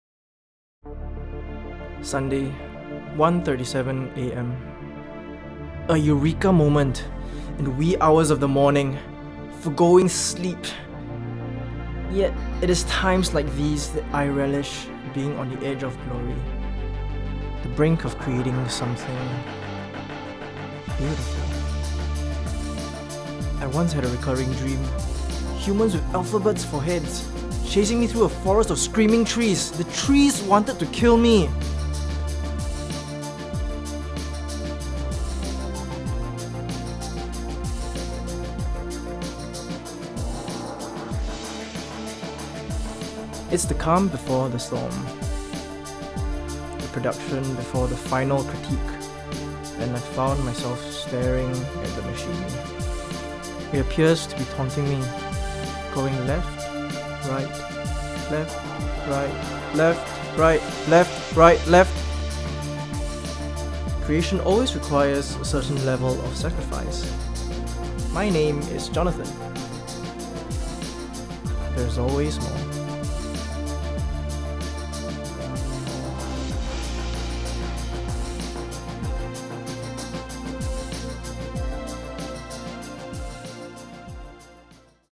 ADM Show 2013 - Visual Communication Promotional Trailer
So what do us students at ADM go through during our 4-year degree programme? Watch to have a glimpse of what happens.

Come join us from the 10th to 19th of May, 2013!